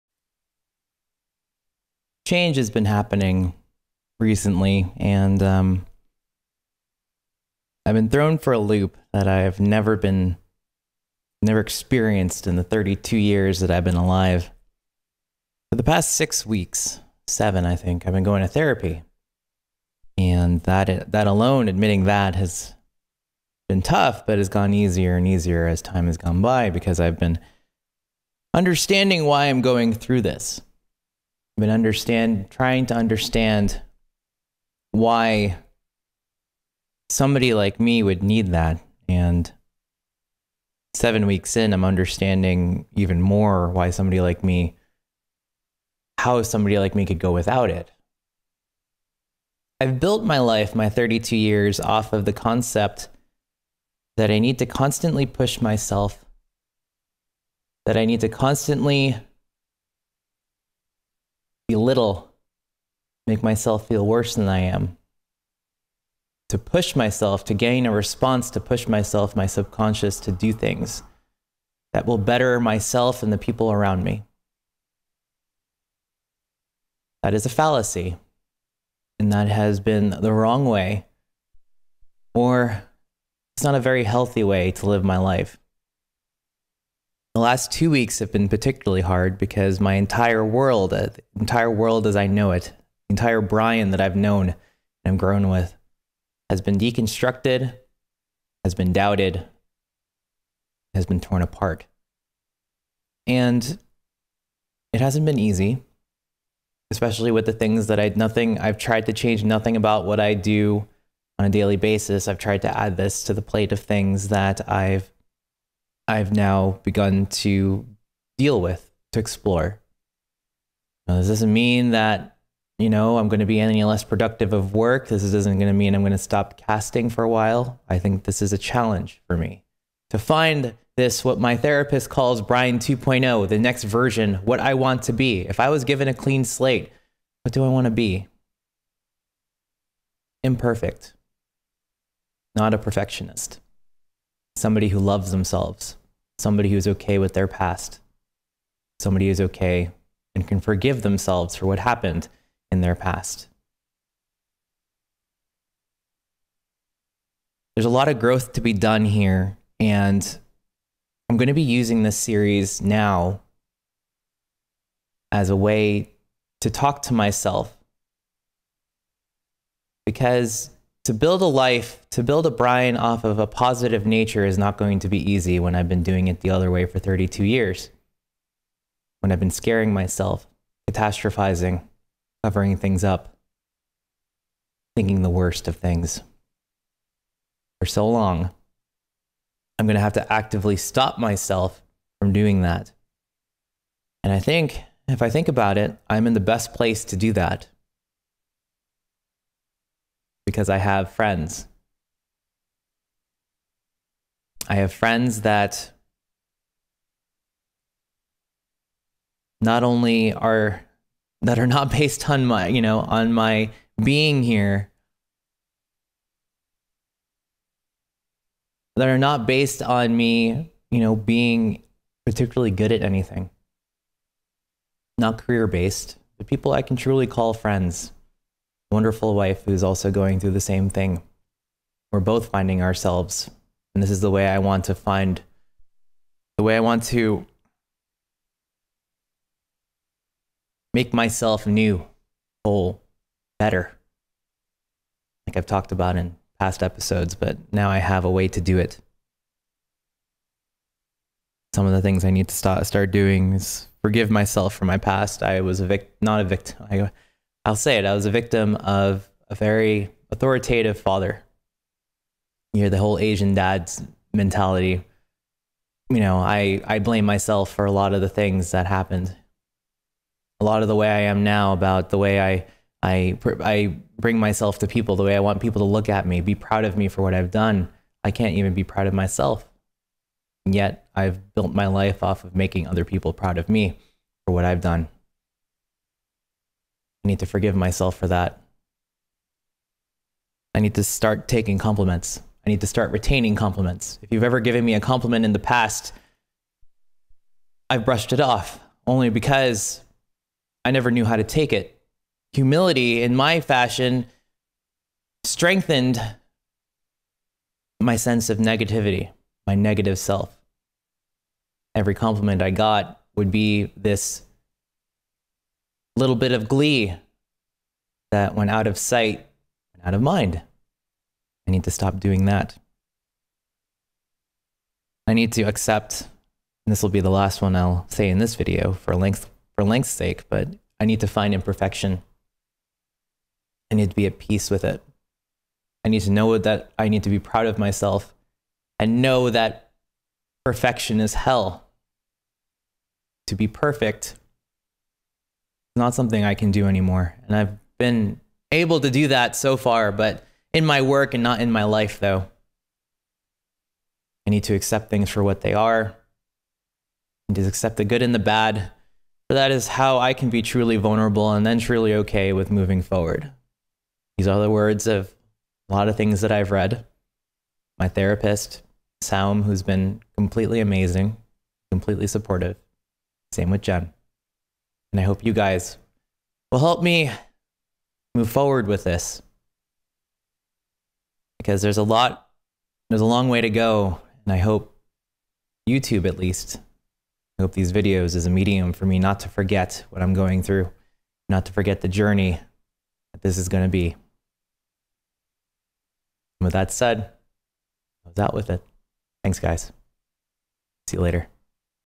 Out With It: Replanning, rebuilding. (#3)
Here lies the third "Out with it."
No cuts, no editing—just rambling.

Admissions are made. Planning needs to be done. I'm going to move forward, as hard as that may sound to me right now.

= Below the fold...
- Bryan's Twitch (Live on weekdays!)